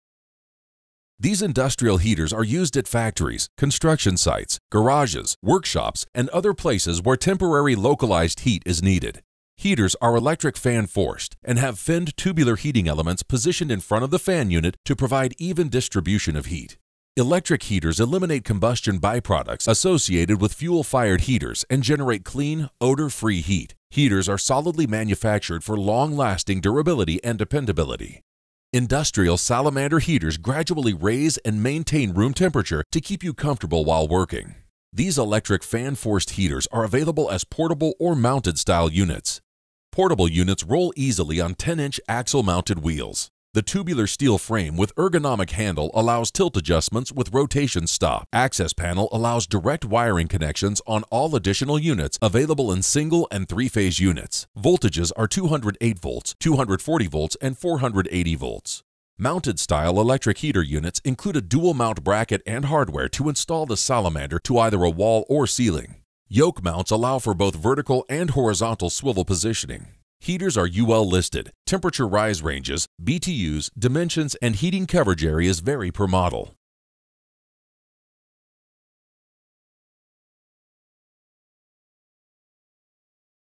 Global Industrial Salamander Electric Heaters
Portable Electric Heaters

Portable Electric Salamander Heaters Provide Temperature Adjustment For Factories, Assembly Rooms, Warehouses, Garages And Other Enclosed Areas.

Portable Electric Salamander Heaters are solidly manufactured for long lasting durability and dependability. Steel venturi housing has finned tubular heating elements to provide steady and secure heat at minimal cost. Tubular steel frame with ergonomic handle allows tilt adjustments with rotation stop. Access panel allows direct wiring connections. Available in Single and 3-Phase Units. Voltages are 208, 240 and 480 volts. Temperature rise ranges, dimensions, and heating coverage areas vary per model.